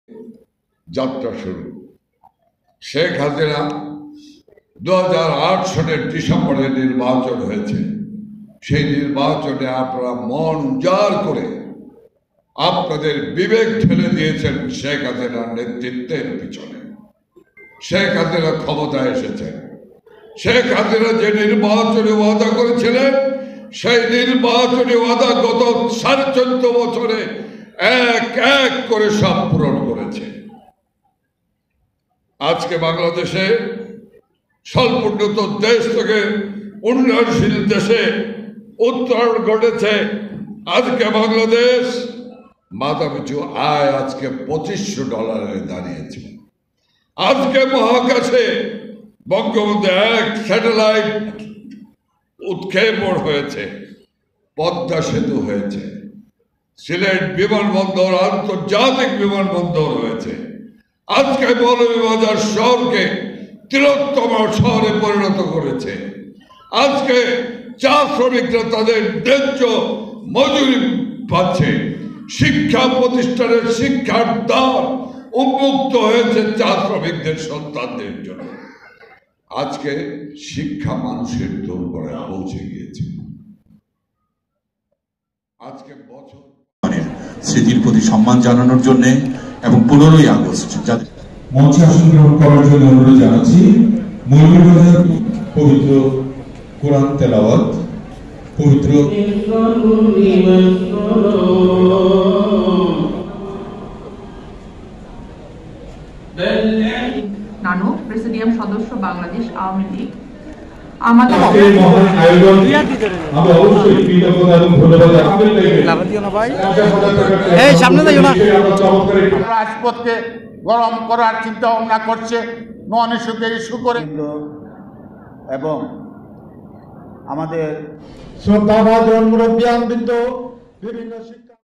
বিগত নির্বাচনে প্রধানমন্ত্রী খে হাসিনা যেসকল ওয়াদা করেছিলেন তার সব পূরণ করেছেন-জাহাঙ্গীর কবির নানক
বিগত নির্বাচনে প্রধানমন্ত্রী খে হাসিনা যেসকল ওয়াদা করেছিলেন তার সব পূরণ করেছেন-জাহাঙ্গীর কবির নানক_MUHIT_BANGLA_NEWS